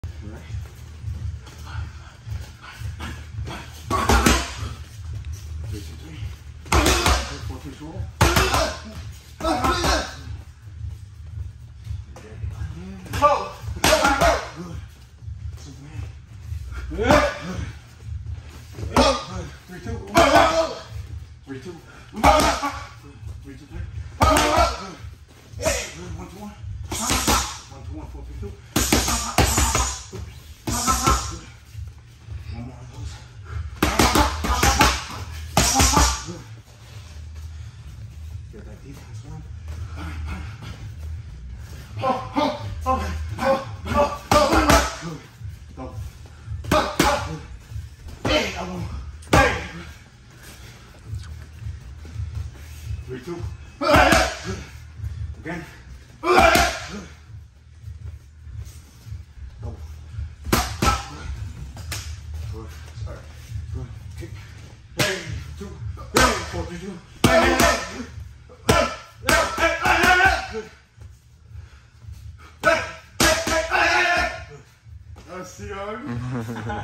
Padwork Session
Friday night Padwork work session. If you can’t make the classes Book your private session. Boxing-kickboxing-MMA-BJJ